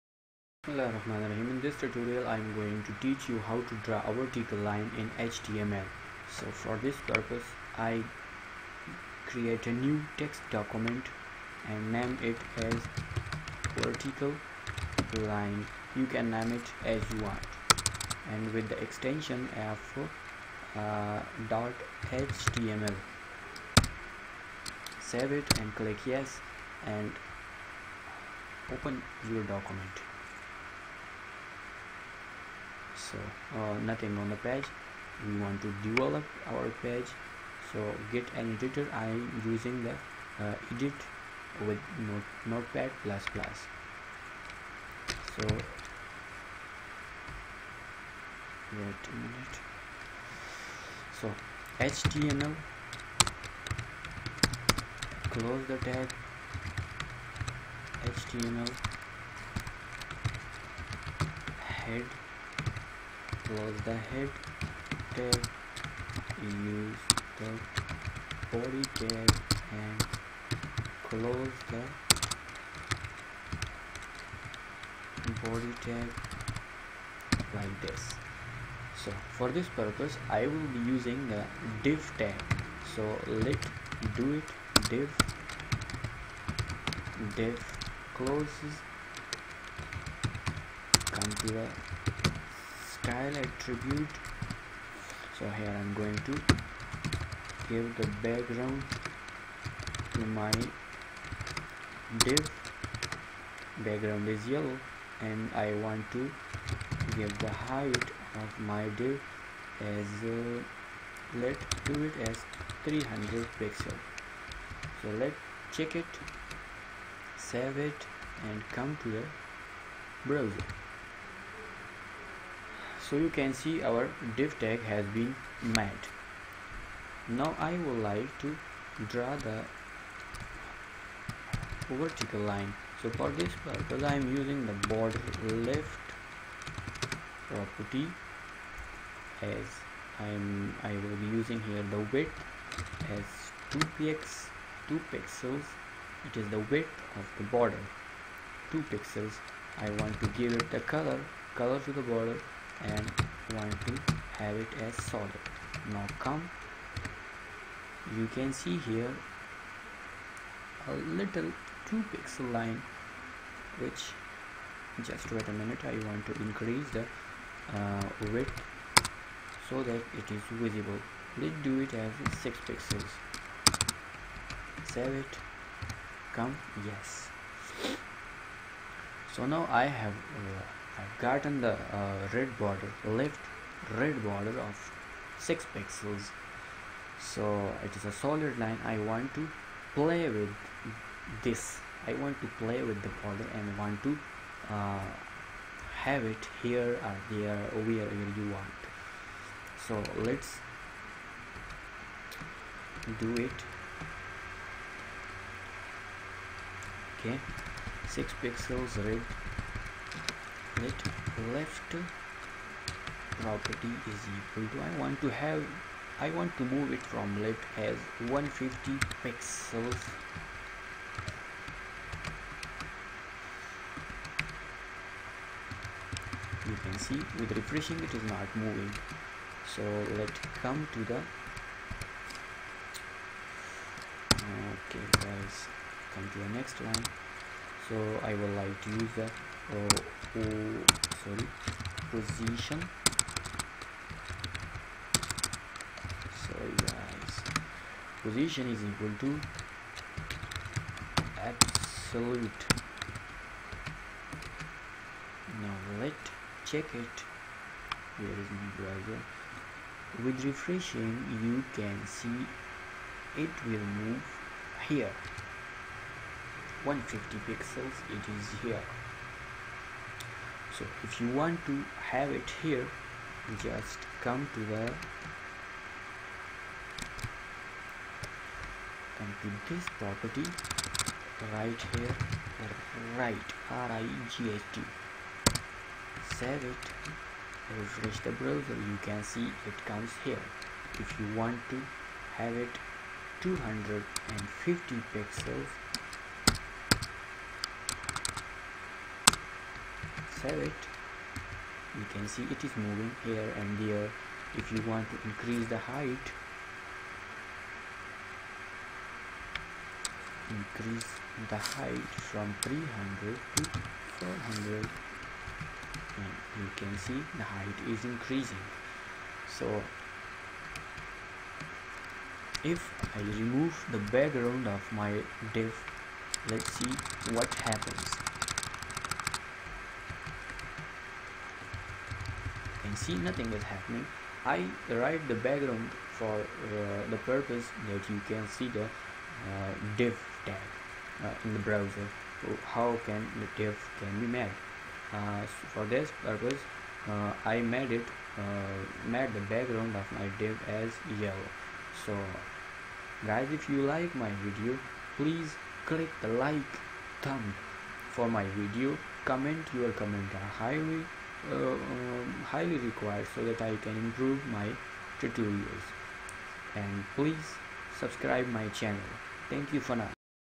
How to draw a vertical line in HTML
html div layout tutorial
html and html5 tutorial

there is no tag for drawing vertical liine in html while horizontal line can easily be drawn with HR tag
I am sorry in advance for my weak english
I just tried my best to make it as simple as i could
Thanks